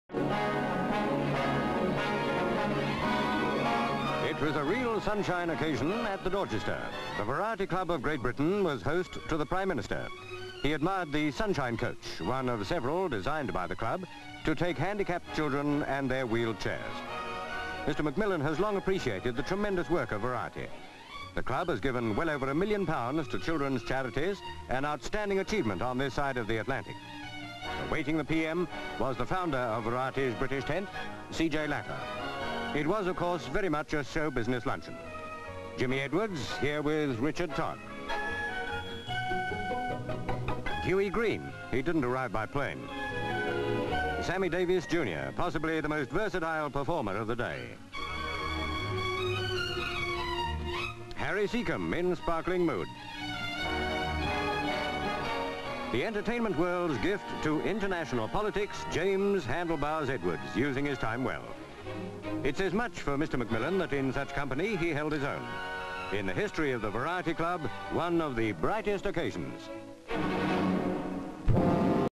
P.M. Meets 'variety' (1963)
Dorchester Hotel, London.

GV. Dorchester Hotel, & LV. LV. Britain's Prime Minster Mr Harold Macmillan looking into 'Sunshine coach' outside Dorchester Hotel on his arrival at Variety Club luncheon. Coach is designed by Variety Club and will be used for transport of handicapped children and their wheelchairs. LV. Prime Minister looking at the bus, showing inside of bus. CU. Mr Macmillan, turns to walk away. GV. Interior, guests gathered for luncheon. SV. Mr Macmillan walks into the reception room. CU. Mr Macmillan. Pan to close up shot of Mr C. J. Latta, Director of Associated British Pathe Corporation (ABPC). SV. Mr Latta talking with Mr Macmillan. SCU. Jimmy Edwards talking to actor Richard Todd. CU. Hughie Green. SCU. Sammy Davis talking to Jimmy Carreras. CU. Sammy Davis Junior. CU. Harry Secombe, radio and TV comedian, dabbing drink behind ears and choking. GV. Guests. CU. Comedian Jimmy Edwards talking with Mr Macmillan. CU. Harry Secombe talking with Sammy Davis Junior. SV. They both burst out laughing.
 FILM ID:1760.18